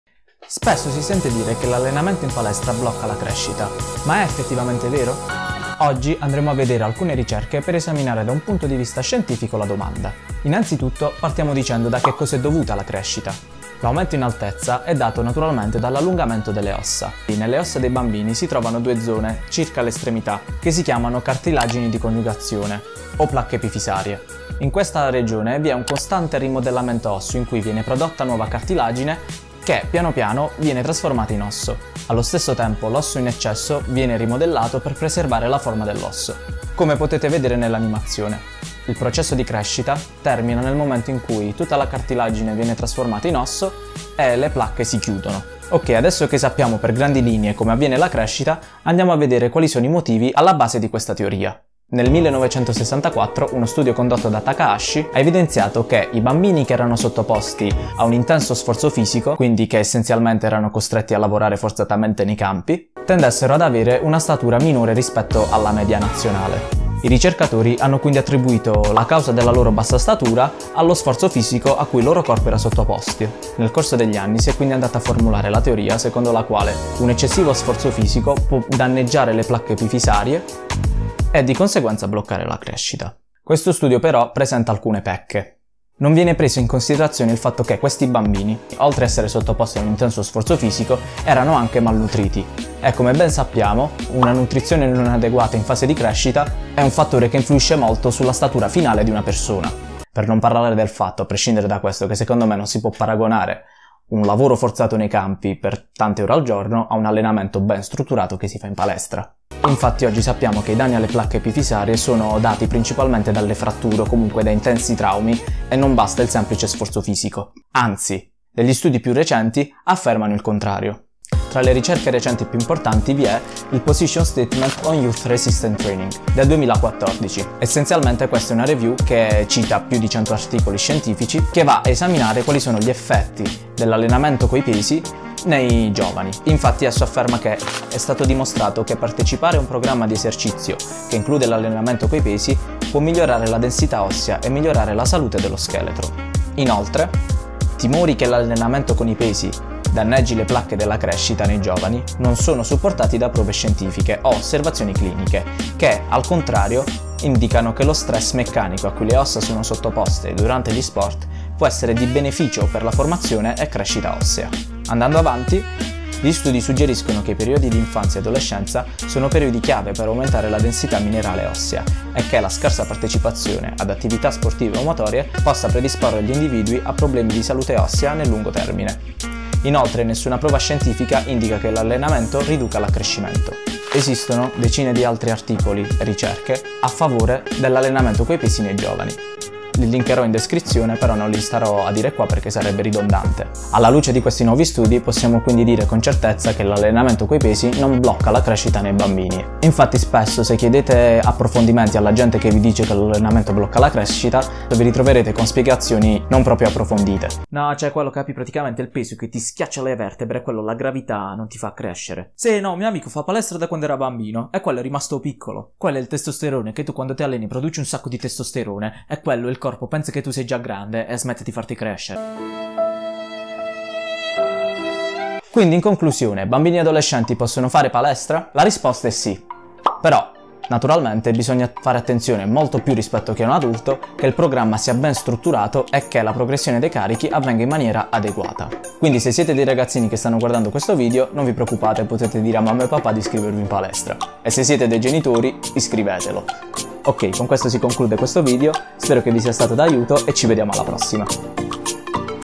IL BODYBUILDING BLOCCA LA CRESCITA?
Si rischia un blocco della crescita sollevando pesi?

Andiamo a scoprire gli effetti che ha l'allenamento con i pesi sulla crescita basandoci sulle ricerche scientifiche più recenti.
Se avete qualche domanda lasciate un commento!